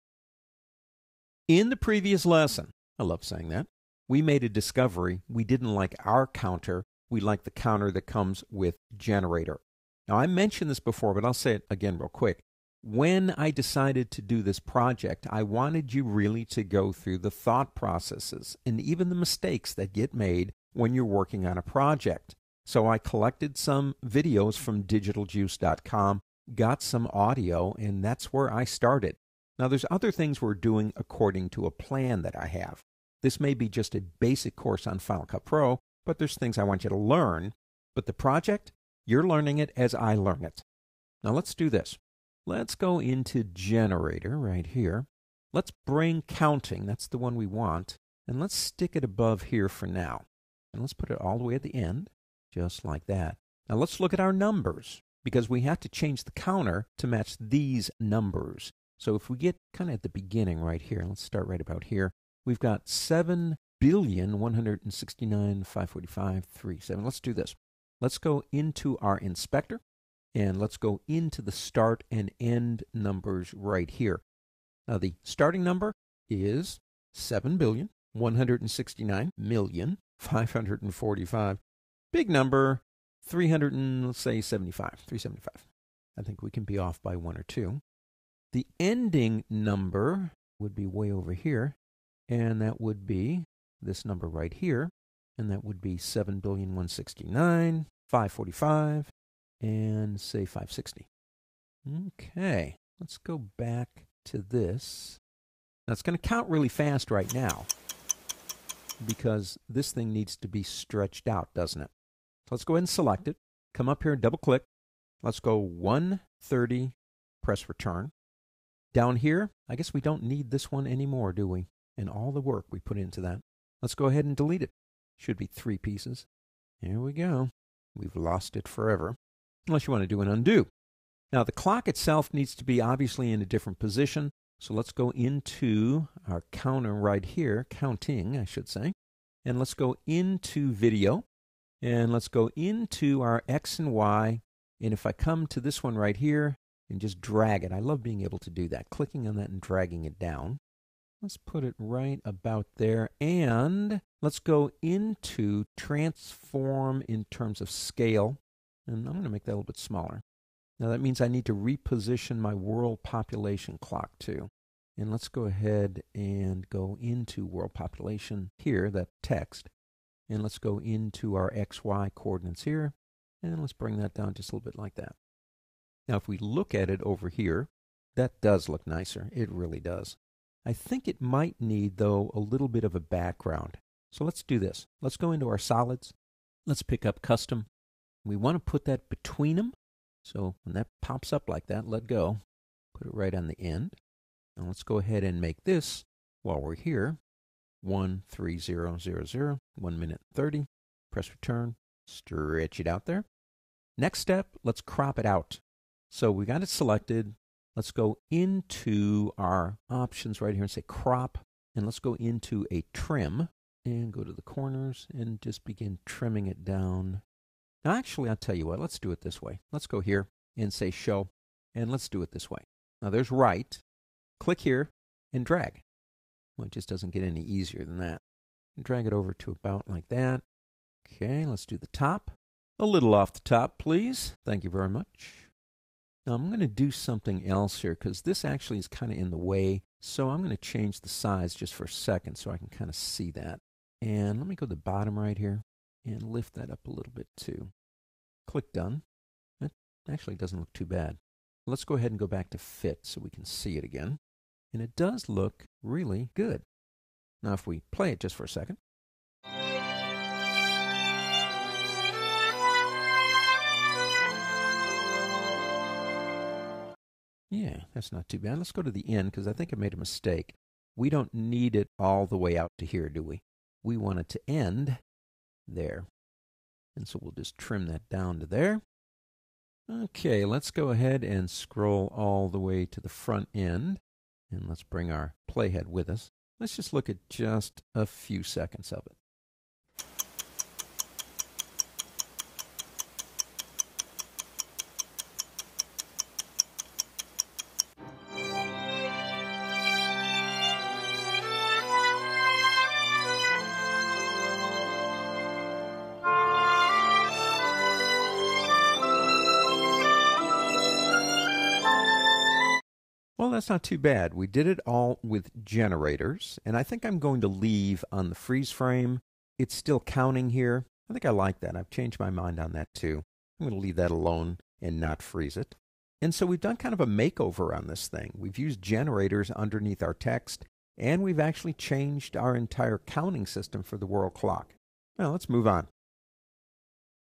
Working With The Counter Generator
He'll guide you, Working With The Counter Generator in Final Cut Pro X!